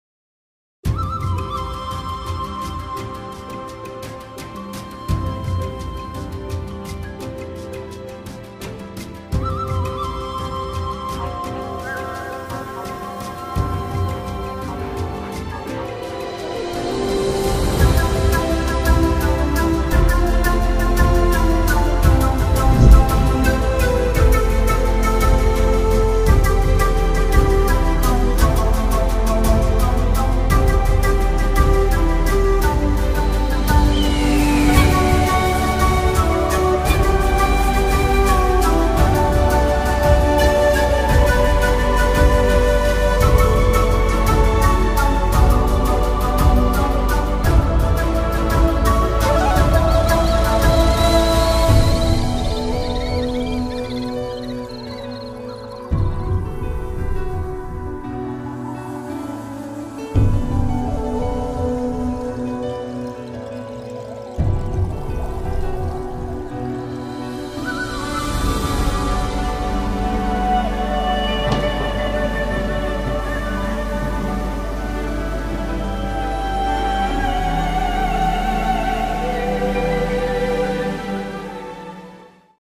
Mici secvente video în drum spre SAHARNA
Secventele video făcute în drum spre Mînăstirea Saharna